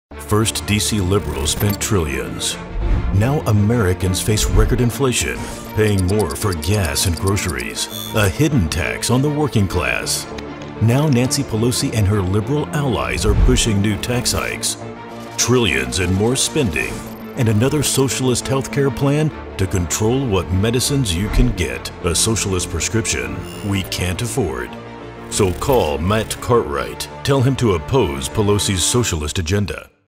Tell Matt Cartwright: Stop Pelosi's Socialist Agenda
Nancy Pelosi's socialist prescription for America includes raising taxes, spending trillions, and a government takeover of your health care by controlling what medicines you can get.